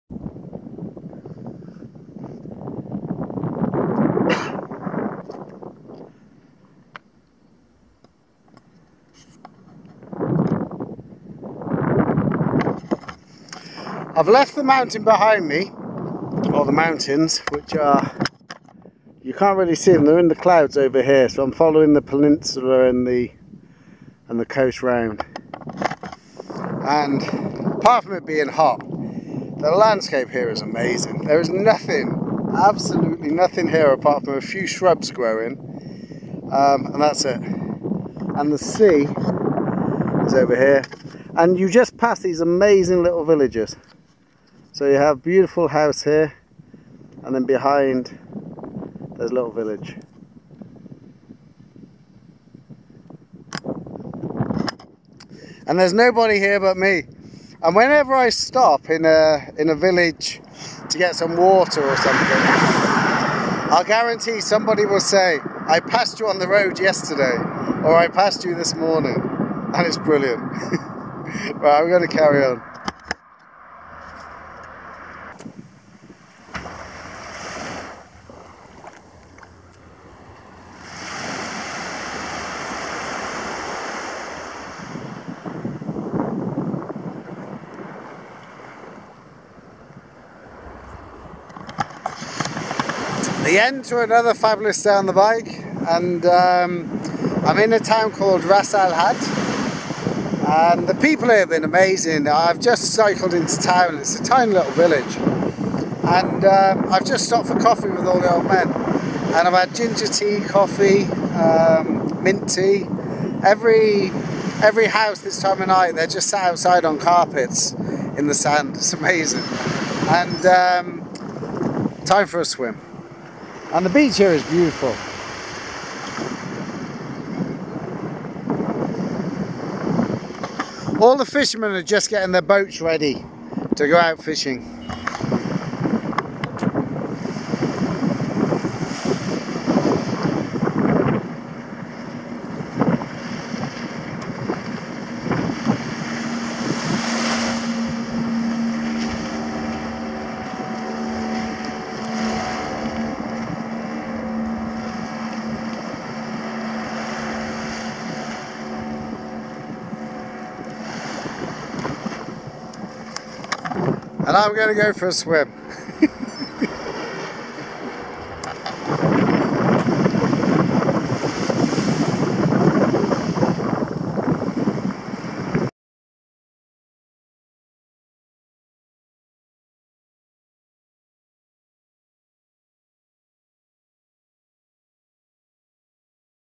The Road To Ras Al Hadd, Oman
The road between Sur and Ras al Hadd is stunning.  Beautiful white sand beaches, empty roads and lots of small fishing villages to sit and enjoy a masala tea.